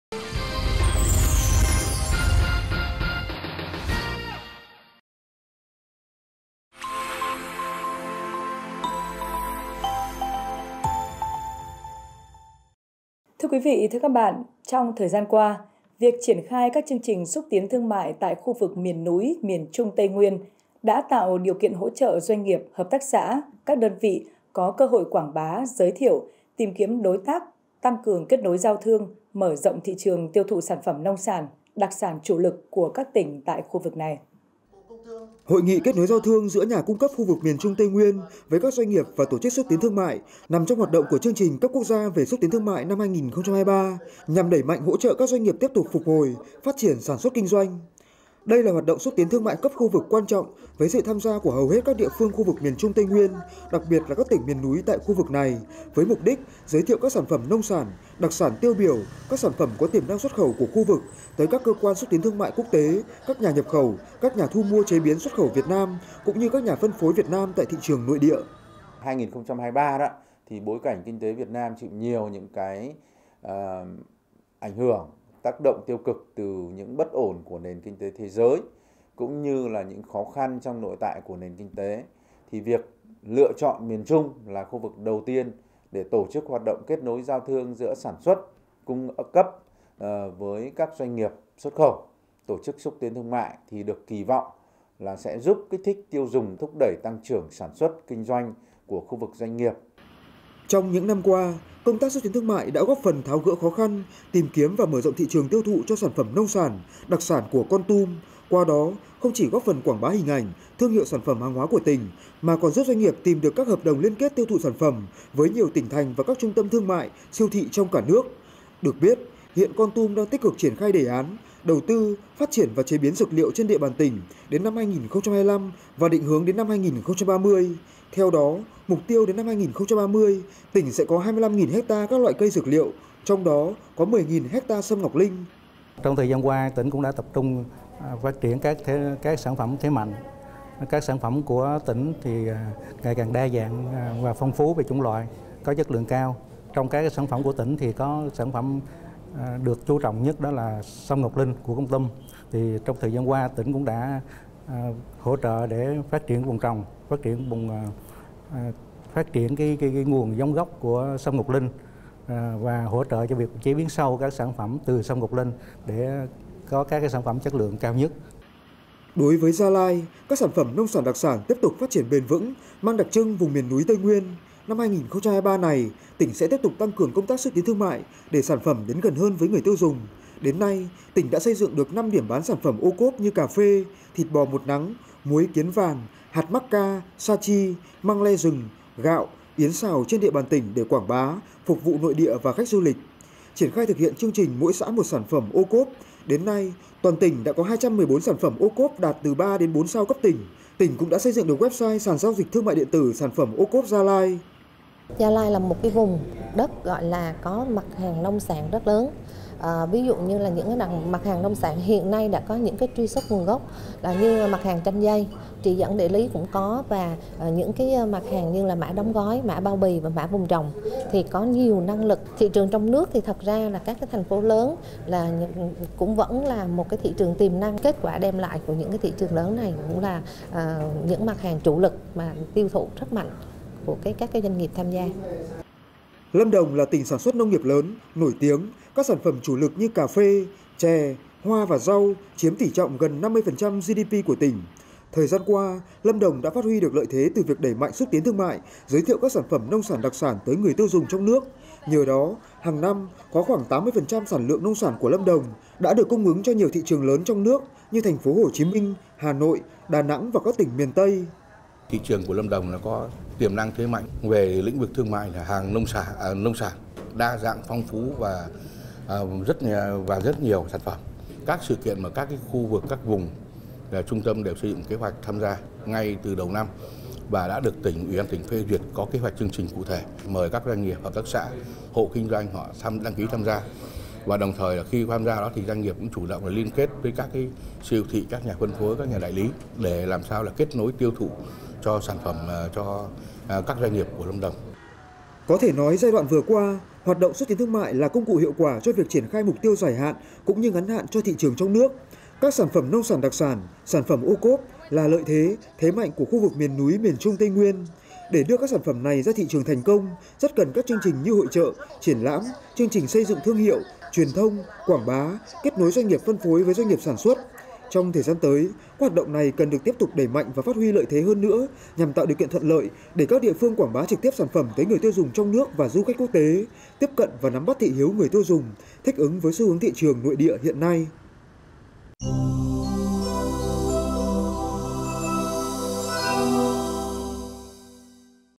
Xúc tiến thương mại nông sản đặc sản miền núi khu vực miền Trung - Tây Nguyên
Trong thời gian qua, việc triển khai các chương trình xúc tiến thương mại tại khu vực miền núi miền Trung - Tây Nguyên, đã tạo điều kiện hỗ trợ doanh nghiệp, hợp tác xã, các đơn vị có cơ hội quảng bá, giới thiệu, tìm kiếm đối tác, tăng cường kết nối giao thương, mở rộng thị trường tiêu thụ sản phẩm nông sản, đặc sản chủ lực của các tỉnh tại khu vực này...